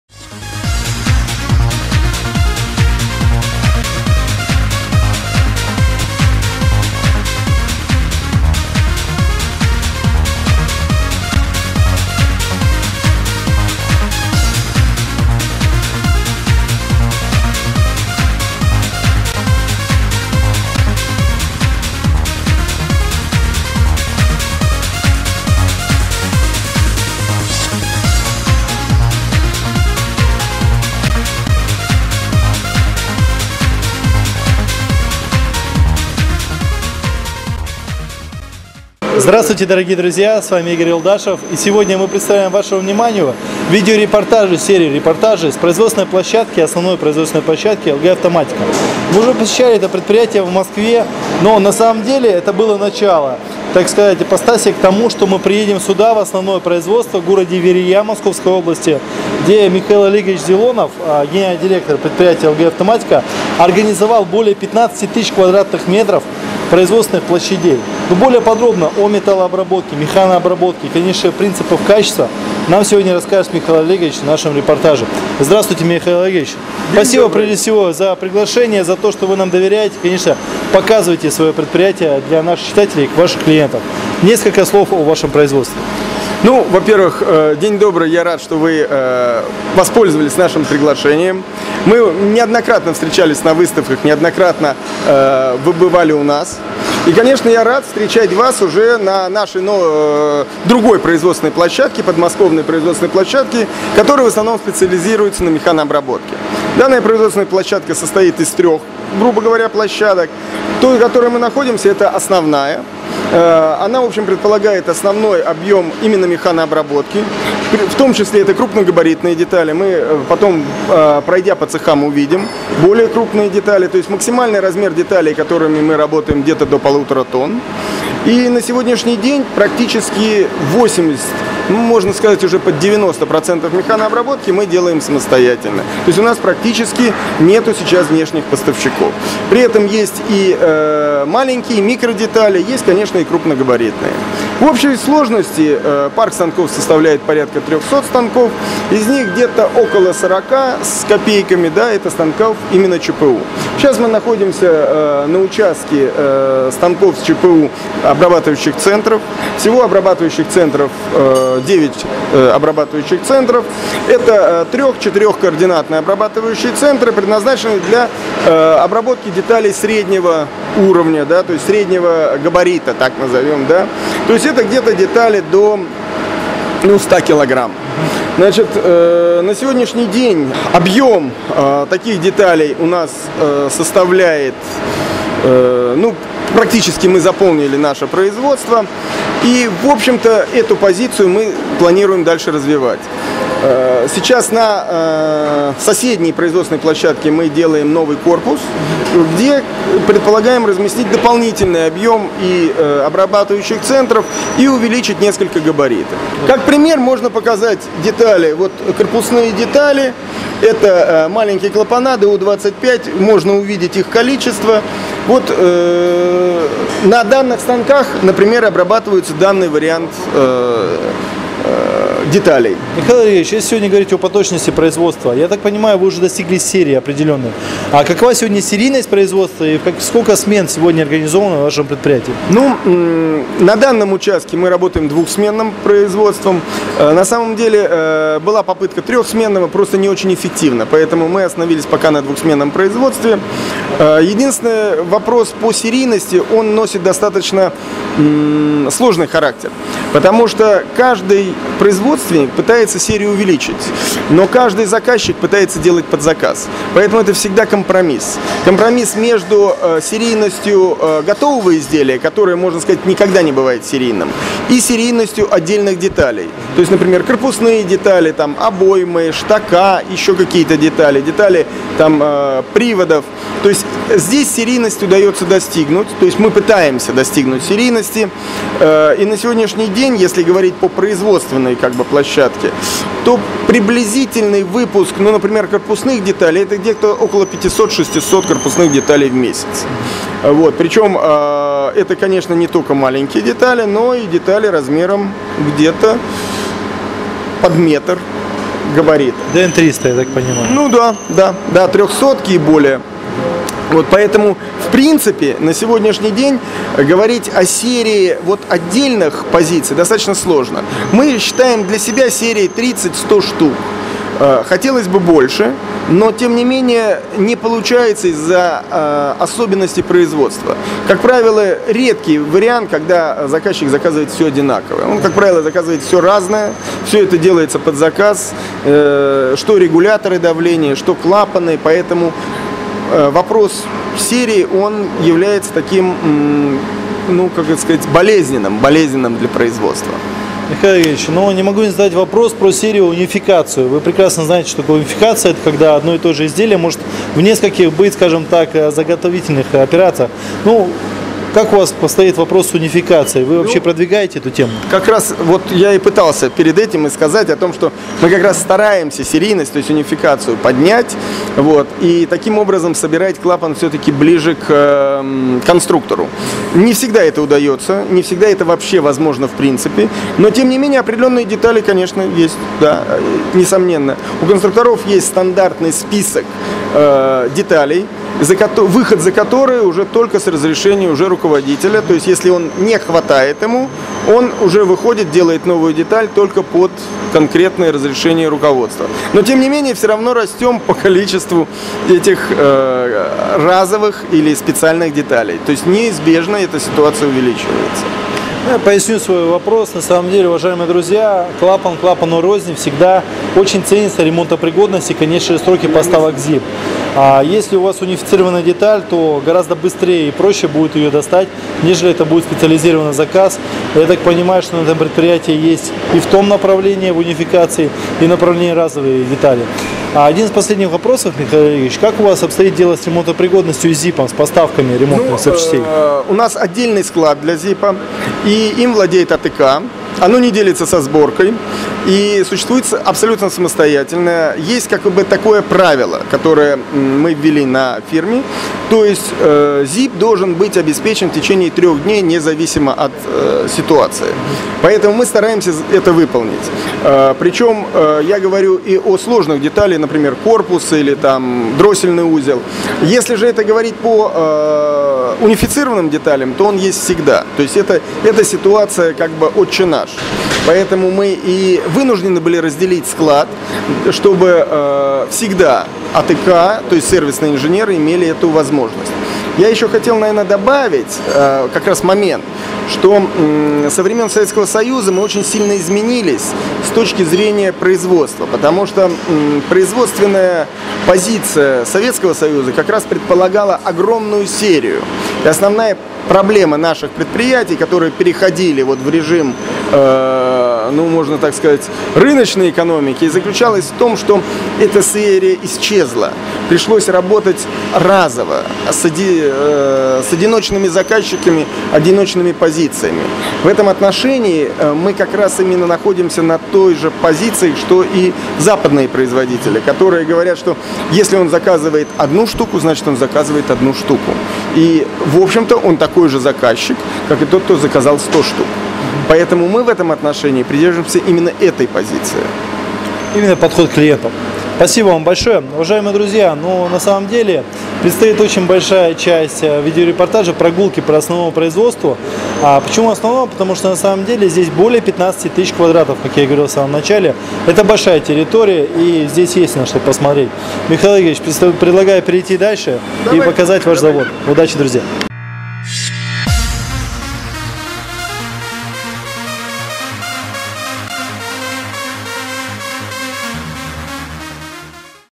«ЛГ автоматика». Металлообработка. Часть I
Добрый день, дорогие друзья и коллеги! Рады сообщить о запуске нового цикла видеорепортажей с производственной площадки. В этот раз мы предлагаем ознакомиться с промышленными возможностями производителя регулирующей трубопроводной арматуры – «ЛГ автоматика». Это предприятие разрабатывает, производит, рассчитывает и поставляет регулирующие, регулирующе-отсечные и отсечные клапаны, в том числе клапаны высокого давления, шланговые, футерованные, клапаны больших диаметров и др. с пневмо, электро и ручным приводами. Разработка и производство всей номенклатуры выпускаемой продукции выполняется на собственной производственной базе.

Начинает цикл репортажей участок, где осуществляется металлообработка. Главный редактор медиагруппы ARMTORG пообщался с генеральным директором предприятия Михаилом Олеговичем Зилоновым.